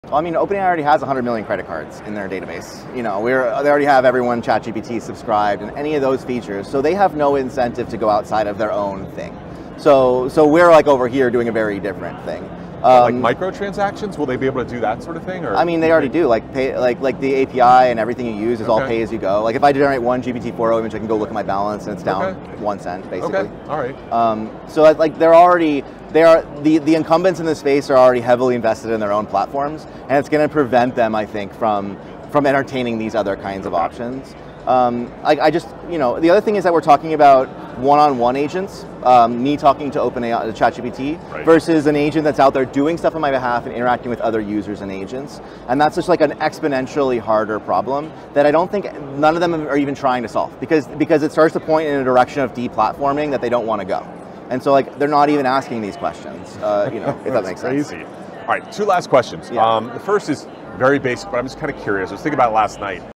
OpenAI's AI Monopoly: Deplatforming & The Future of Agents
Shaw Walters with ElizaOS or Eliza Labs - We discuss why OpenAI's massive user base and platform lock-in may hinder innovation in AI agents and deplatforming solutions. One-on-one agent interaction is an exponentially harder problem to solve.